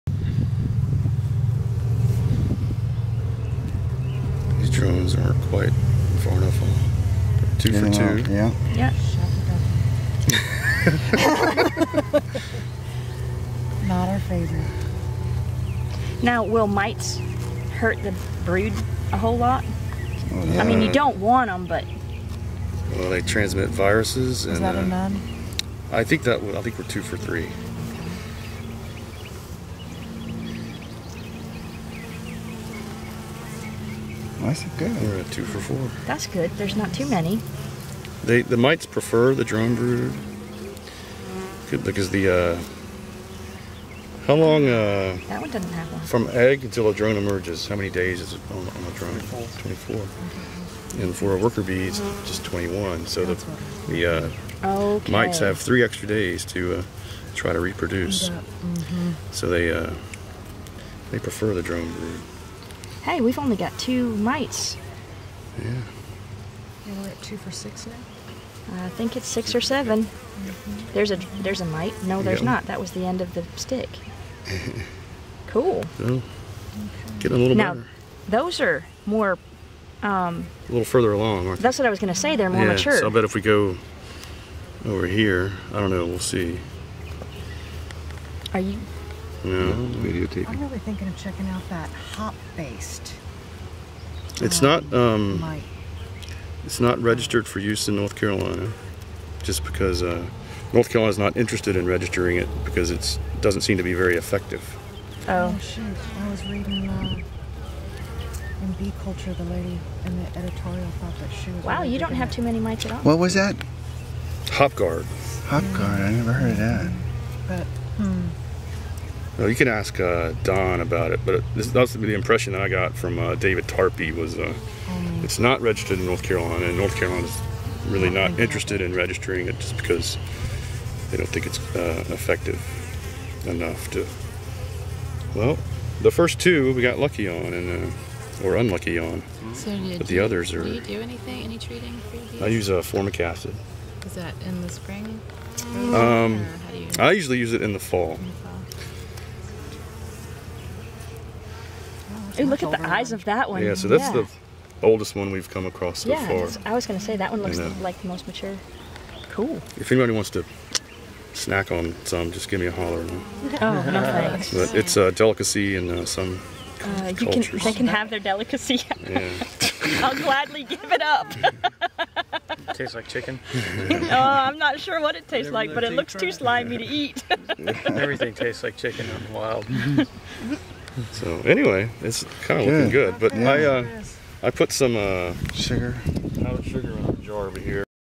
Varroa mite testing on drone brood
Lewis of the Orange County Beekeepers Association demonstrating how to pull drone brood to check for mites in the brood.